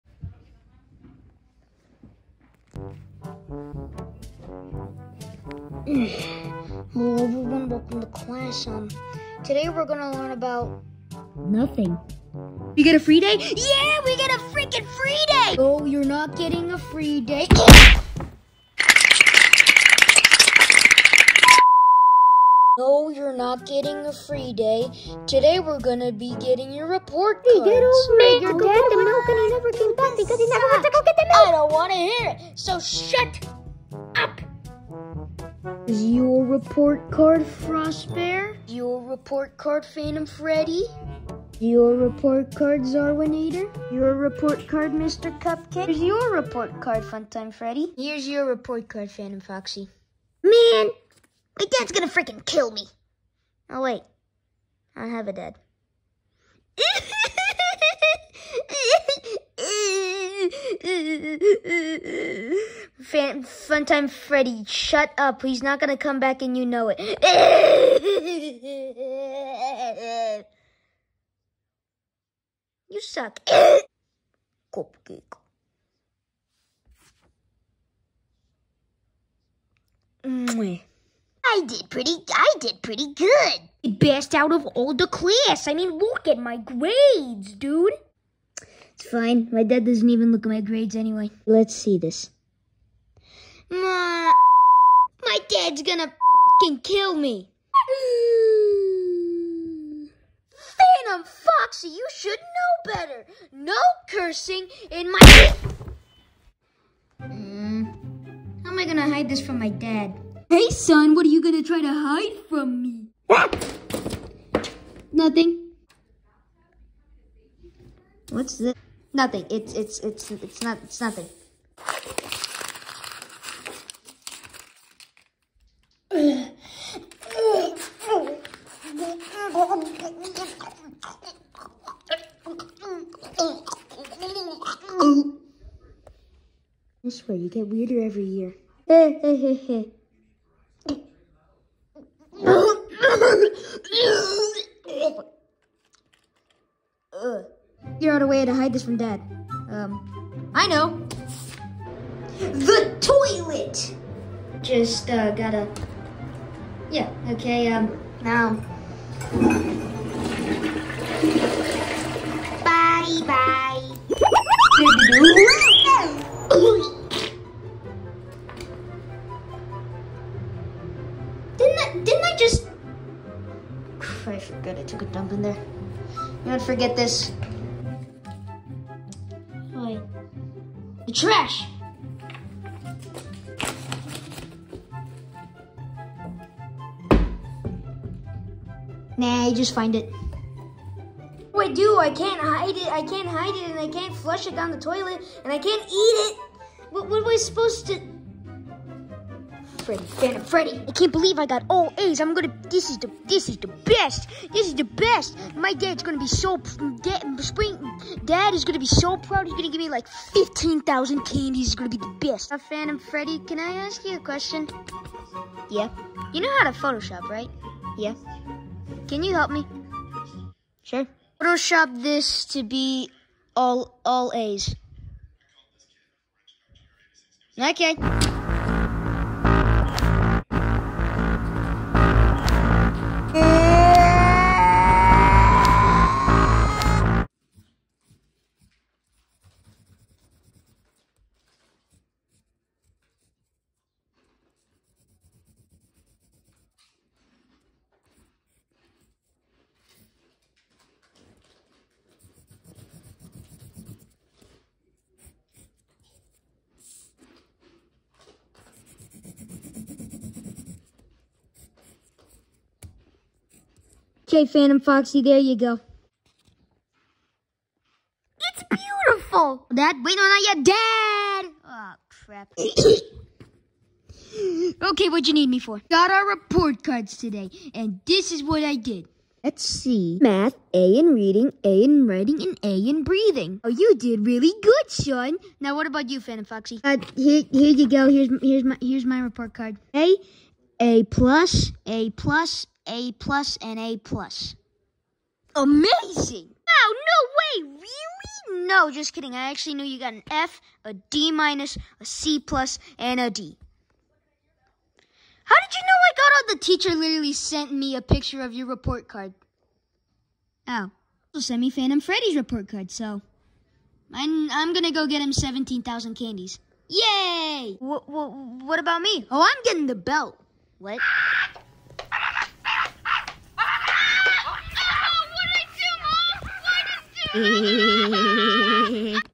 CBMatteo movie (the report card problem)
fnaf fivenigjtsatfreddys funny phantom foxy gets bad grades and try’s his best to hide it from springtrap